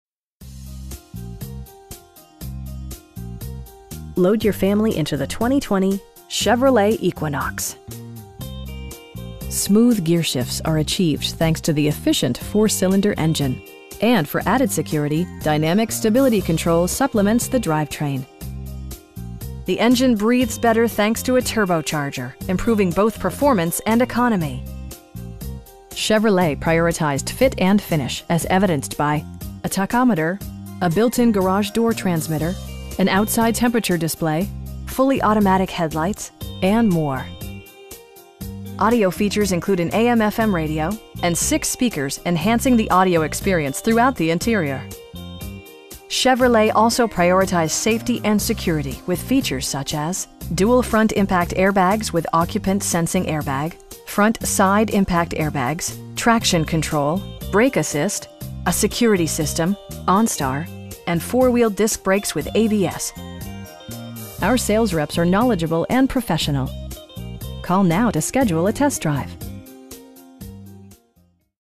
2020 Chevrolet Equinox LT in Tallahassee, FL 32304
Dale Earnhardt Jr. Chevrolet
3127 West Tennessee Street

Load your family into the 2020 Chevrolet Equinox. Smooth gearshifts are achieved thanks to the efficient 4 cylinder engine, and for added security, dynamic Stability Control supplements the drivetrain. The engine breathes better thanks to a turbocharger, improving both performance and economy. Chevrolet prioritized fit and finish as evidenced by: a tachometer, a built-in garage door transmitter, an outside temperature display, fully automatic headlights, telescoping steering wheel, cruise control, and more. Audio features include an AM/FM radio, and 6 speakers, enhancing the audio experience throughout the interior. Chevrolet also prioritized safety and security with features such as: dual front impact airbags with occupant sensing airbag, front side impact airbags, traction control, brake assist, a security system, onStar, and 4 wheel disc brakes with ABS. Our sales reps are knowledgeable and professional. We'd be happy to answer any questions that you may have.